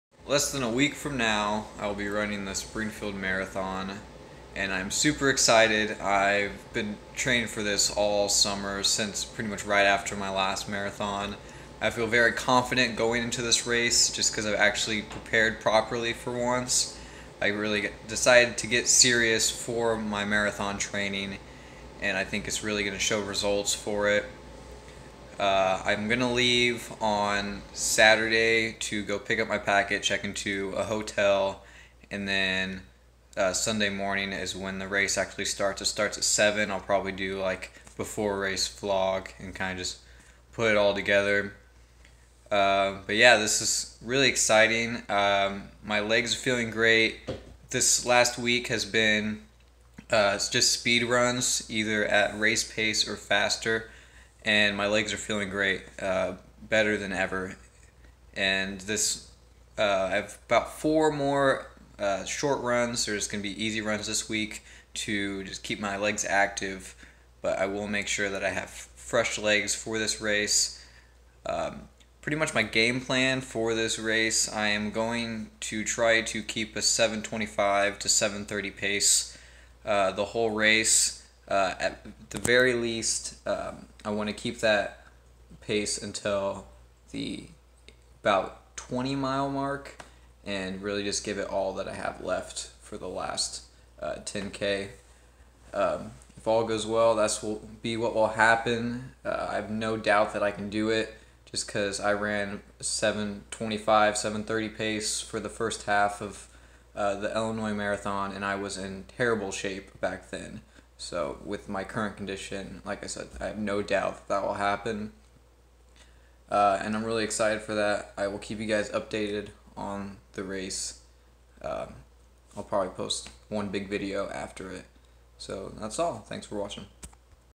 My Race Plan for the Springfield Marathon.
Less than one week left before this race and I'm confident about my race goals. I will be giving it all that I have and I think my training has prepared me well. Also I said the race starts at 7, its actually 7:30.